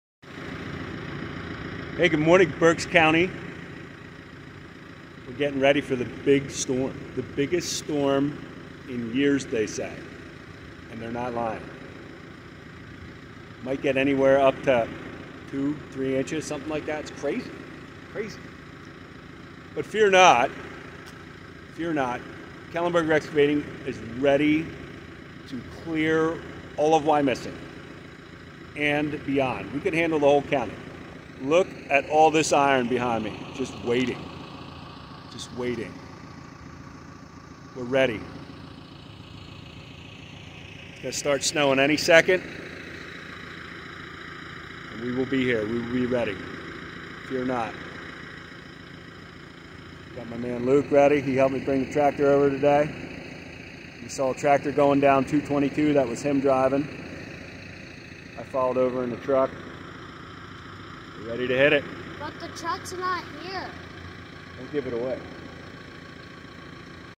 Snow Prep
KX is ready to move some snow.  after a multi-year hiatus of commercial snow removal we are back at it.